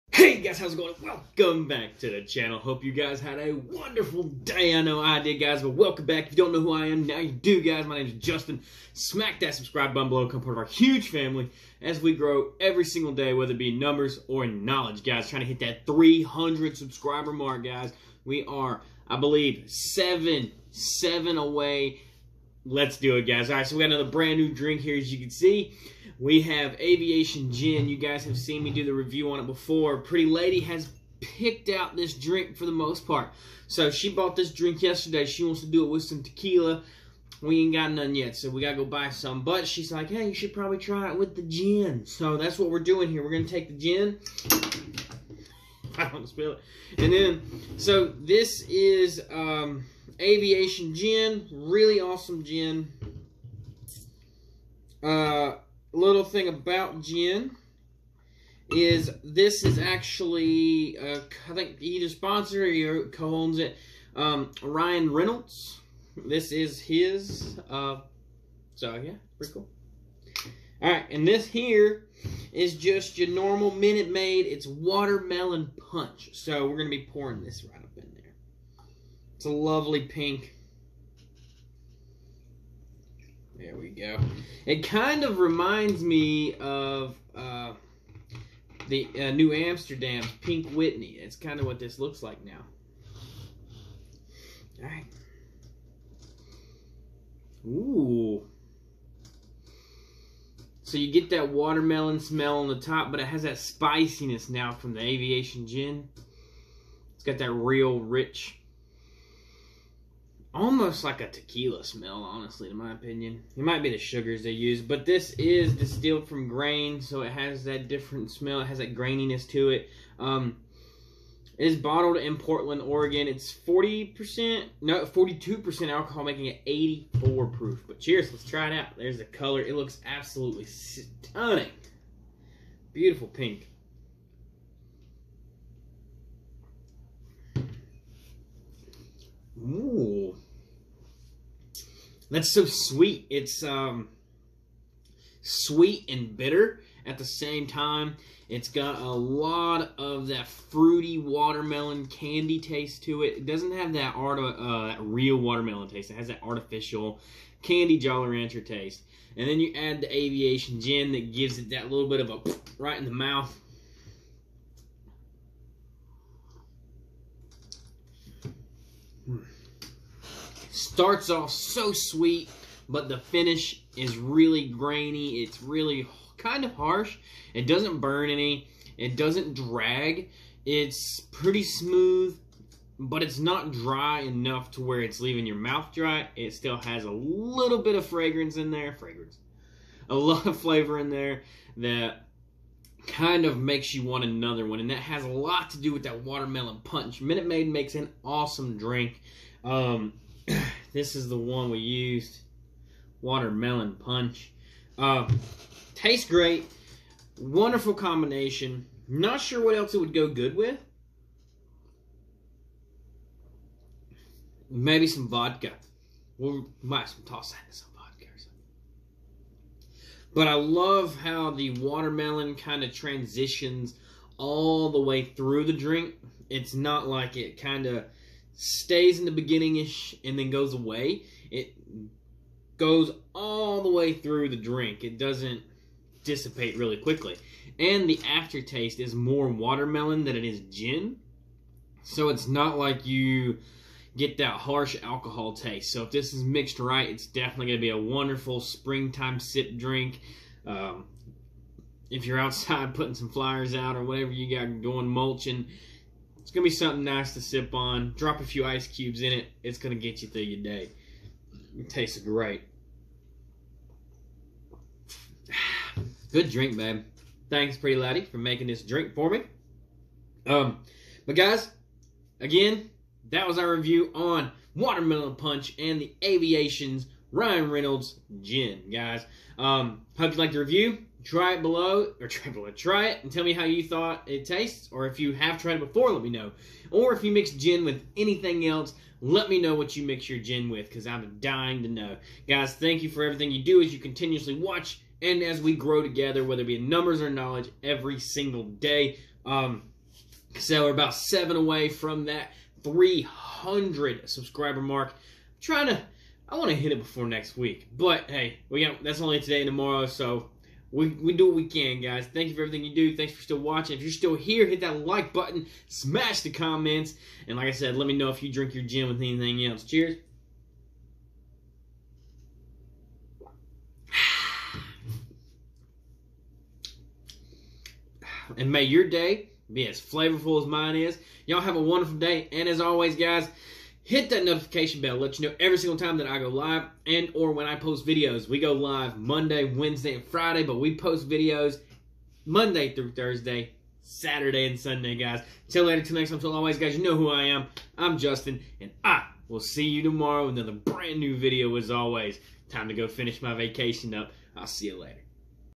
Aviation Gin + Watermelon Punch = The Only Punch
What An Amazing Mix Thanks To The Pretty Lady! Also Learned that Ryan Reynolds Sold His Part Of The Drink!